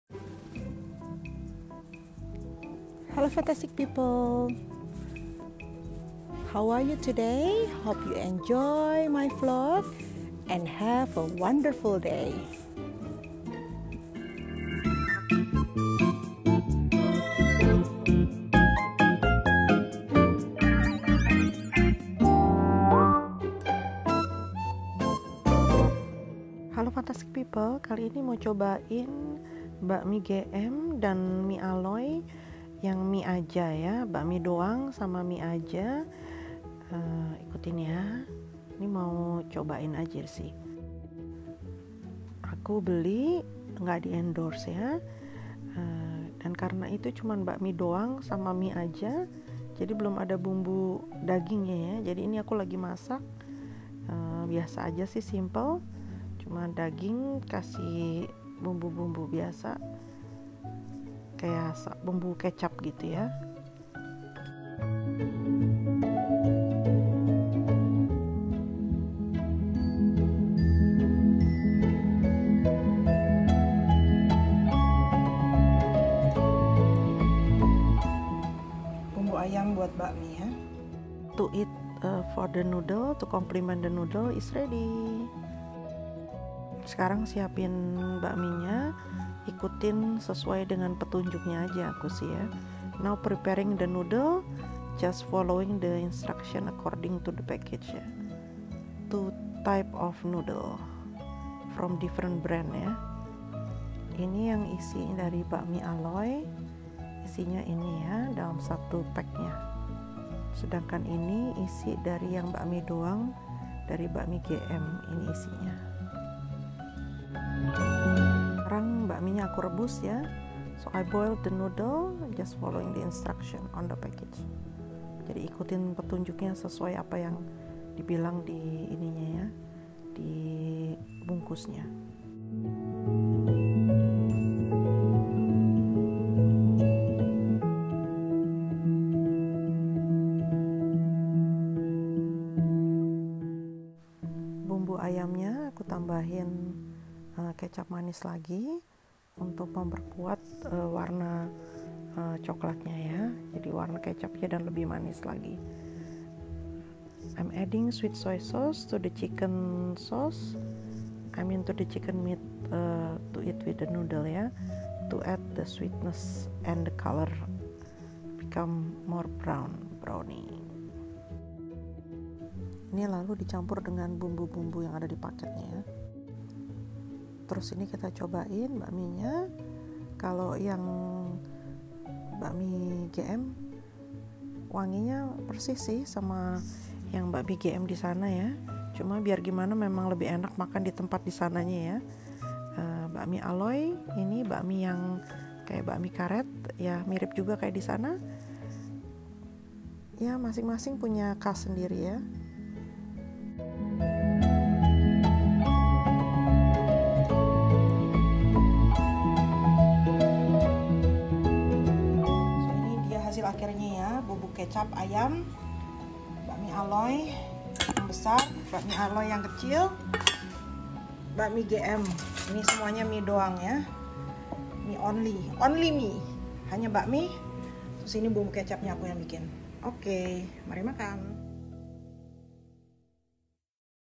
Review cobain masak bakmie Gm-bakmi doank & Mie Aloi-mie aja dimakan pakai bumbu ayam kecap homemade
Hi fantastic people❤️
Vlog pendek aja pas review cobain bakmie Gm-bakmi doank & Mie Aloi-mie aja
dimakan pakai bumbu ayam kecap homemade buatan sendiri.